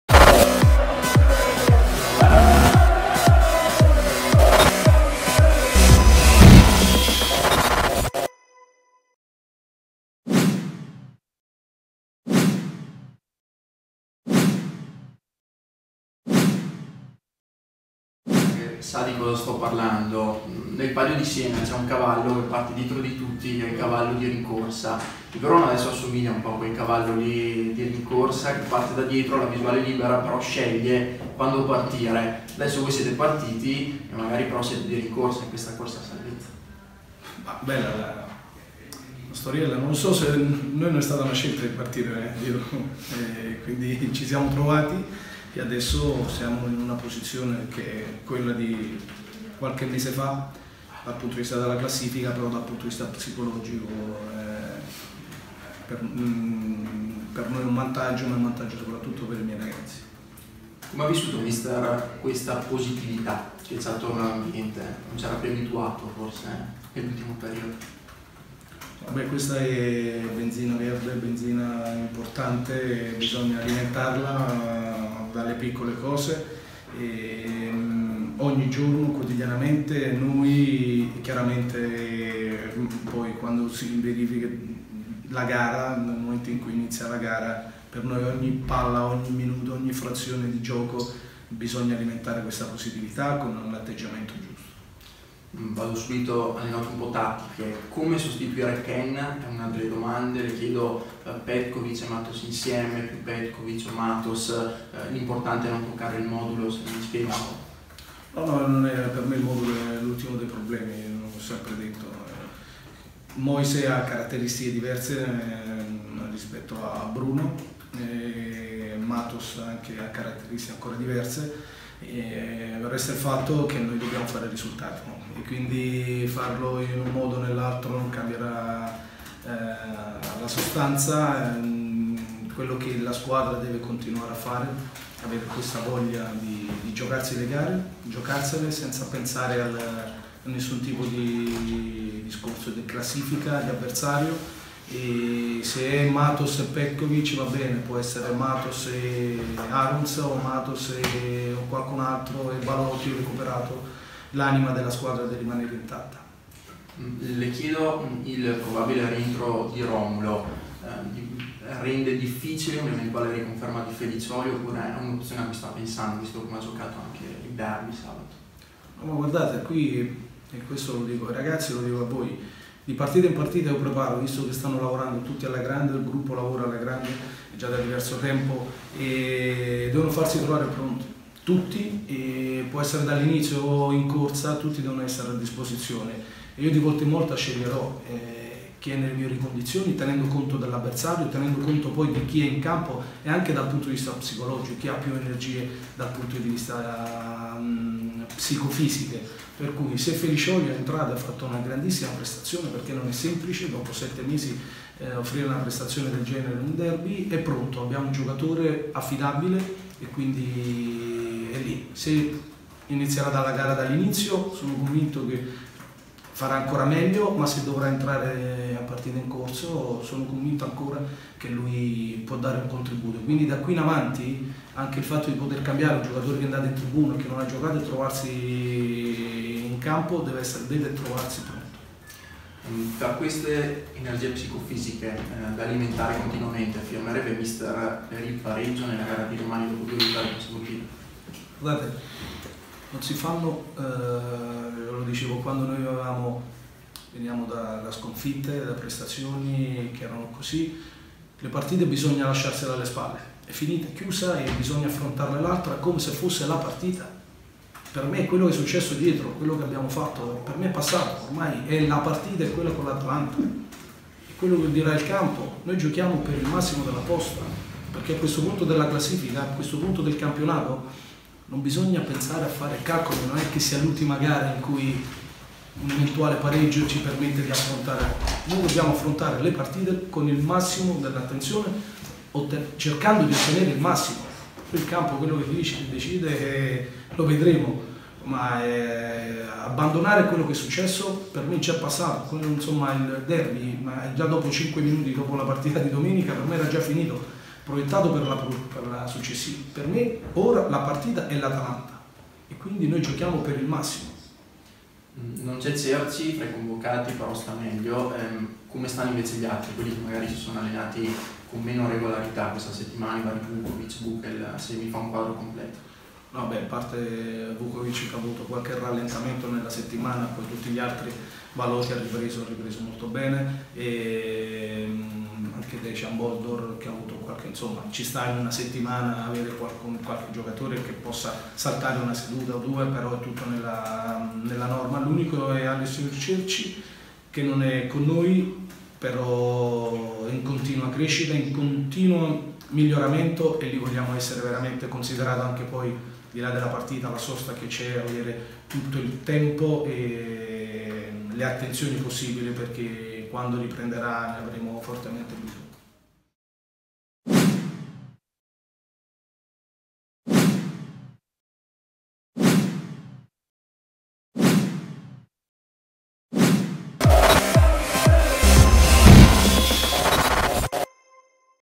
Pecchia: ”Cerci è in continua crescita e miglioramento”
Vigilia di campionato per l’Hellas Verona. L’allenatore gialloblù parla dell’Atalanta, dei giocatori recuperati e della condizione di Alessio Cerci